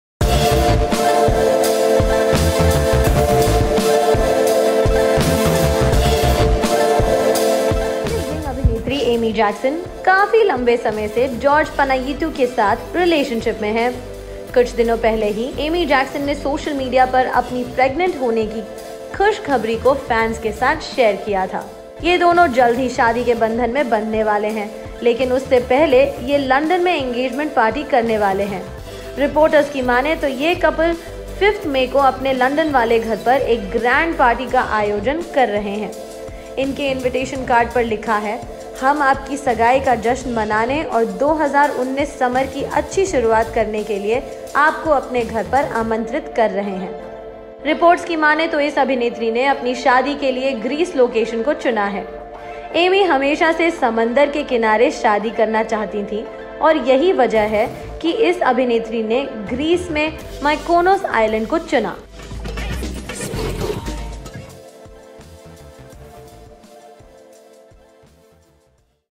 Pregnant Amy Jackson Is Marrying Boyfriend George Panayiotou On THIS Date
Actress AmyJackson has been dating GeorgePanayiotou for a long time now. She shared the news of her pregnancy on social media just a few days ago. Now both Amy & George are ready to tie the knot on the 5th of May,2019 according to reports.